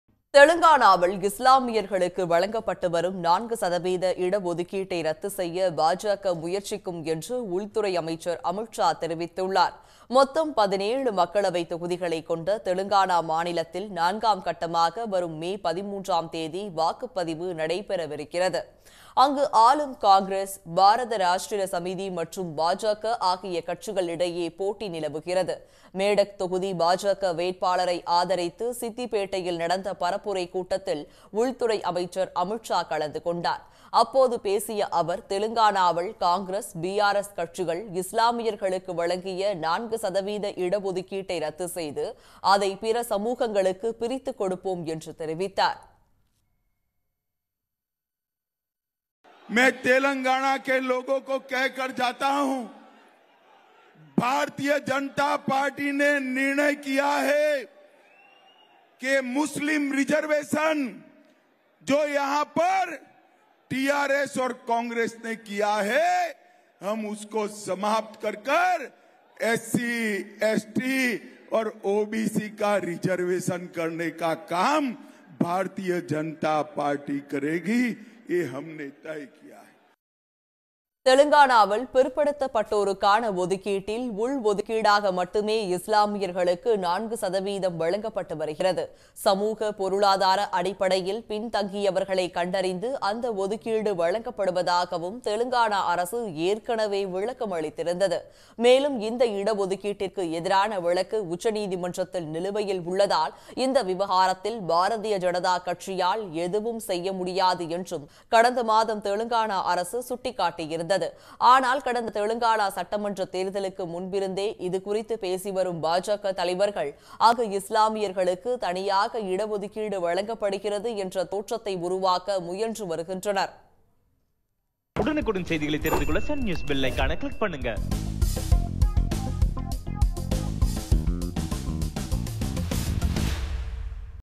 தெலுங்கானாவில் 4% இஸ்லாமியர் இடஒதுக்கீட்டை ரத்து செய்வோம் -அமித்ஷா | Telangana | Election | Amit Sha
தெலுங்கானாவில் 4% இஸ்லாமியர் இடஒதுக்கீட்டை ரத்து செய்வோம் -அமித்ஷா | Telangana | Election | Amit Sha | Sun News